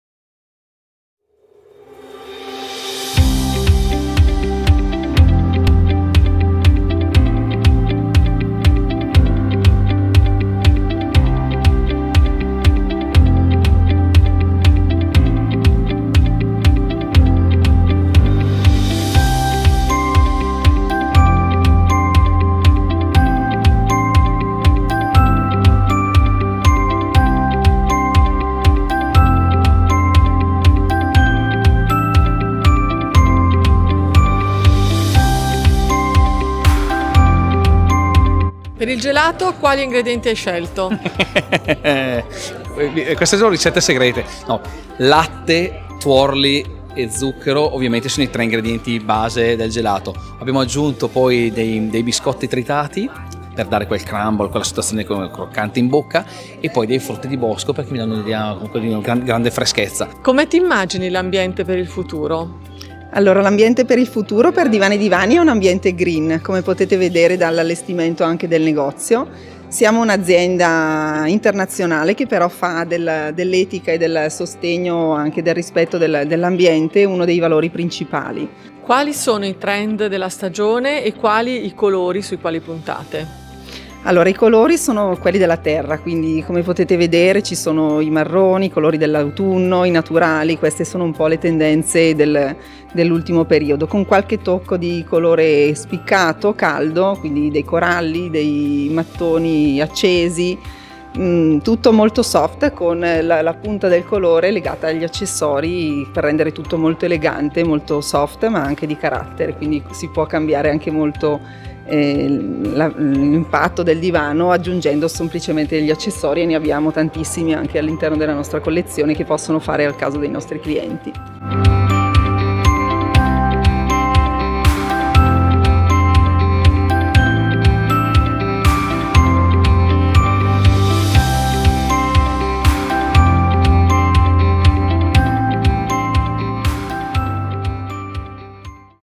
Divani & Divani Bergamo - Evento green con Chef Mainardi
Look total green per lo store Divani & Divani di Bergamo pronto ad accogliere i clienti con la cucina dello Chef Mainardi e green gadgets.